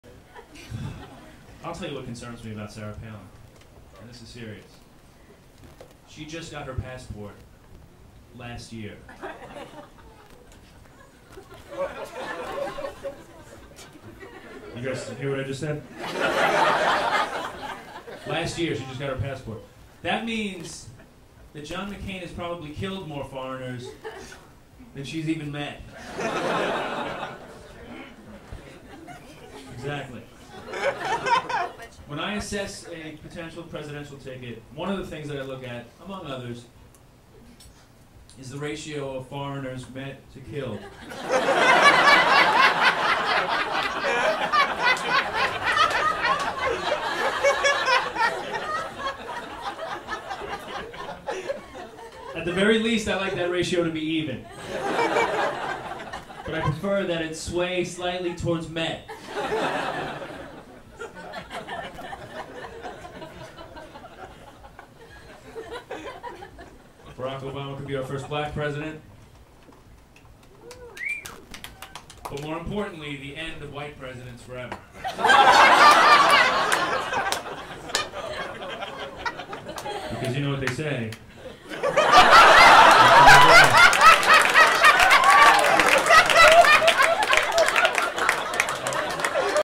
"President Obama & Sarah Palin"- Ted Alexandro Stand Up
Ted Alexandro talks about Barack Obama & Sarah Palin.

Live Streaming 2x a week, Ted talks on ALL the topics!